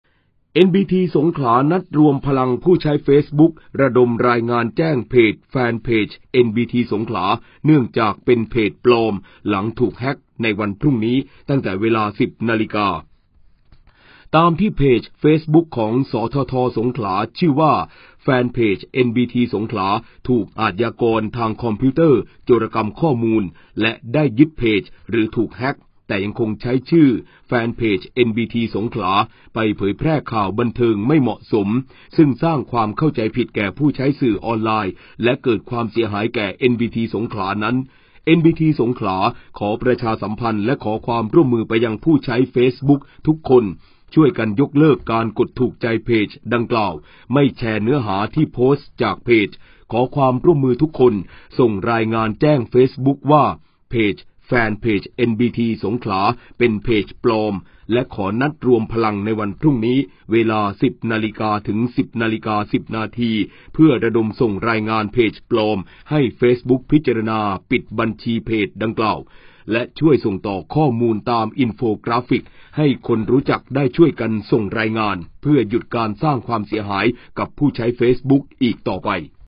NBT สงขลา นัดรวมพลังผู้ใช้เฟซบุ๊ก ระดมรายงานแจ้ง เพจ Fanpage NBT Songkhla เนื่องจาก เป็นเพจปลอม
NBT สงขลา นัดรวมพลังผู้ใช้เฟซบุ๊ก ระดมรายงานแจ้ง เพจ Fanpage NBT Songkhla เนื่องจาก เป็นเพจปลอม หลังถูกแฮค ในวันพรุ่งนี้ ตั้งแต่ เวลา 10.00 น. ออกอากาศวันที่ 29 ม.ค. 66 ภาค 17.00 น. โดย สวท.กทม.